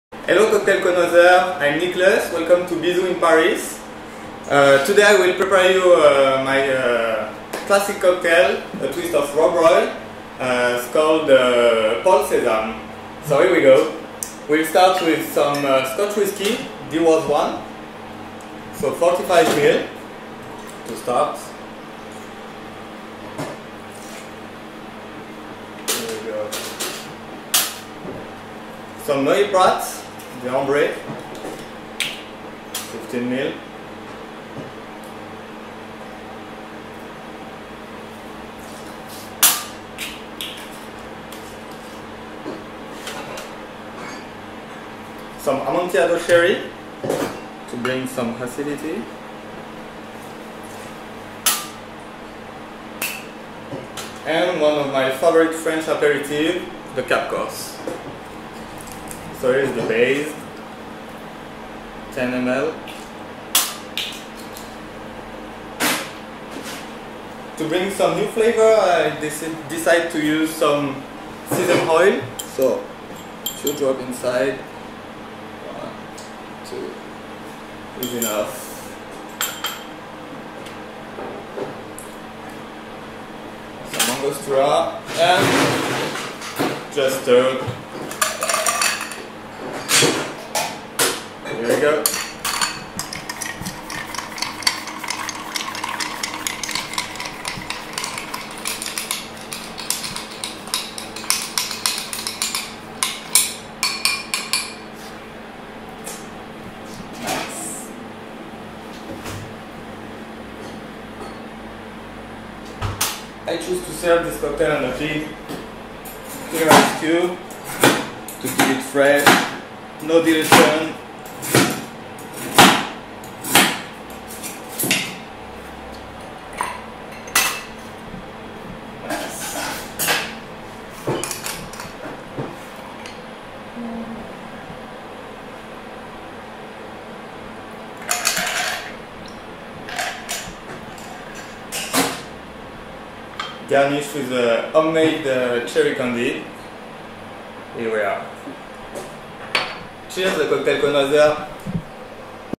Nicolas Goradesky from Bisou (Paris): Paul Sesame
Nicolas Goradesky from Bisou makes for us his twist on a classic Rob Roy the Paul Sesame